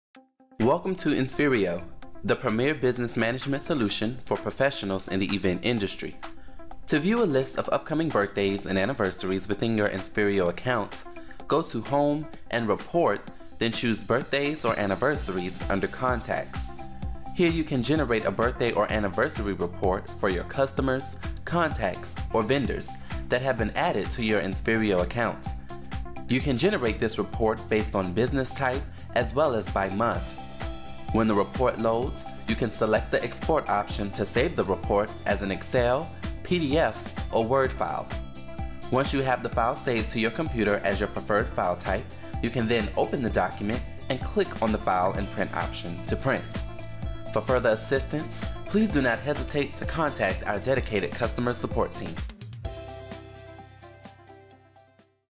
Birthdays/Anniversaries - Reports
Welcome to Inspherio, the premier business management solution for professionals in the event industry.

To view a list of upcoming birthdays and anniversaries within your Inspherio account, go to Home and Report, then choose Birthdays/Anniversaries under Contacts.

Here you can generate a Birthday or Anniversary report for your Customers, Contacts or Venders that have been added to your Inspherio account.

You can generate this report based on business type as well as by month.

When the report loads you can select the export option to save the report as an excel, word or pdf file.

Once you have the file saved to your computer as your preferred file type, you can then open the document and click on the file and print option to print.

For further assistance, please do not hesitate to contact our dedicated customer support team.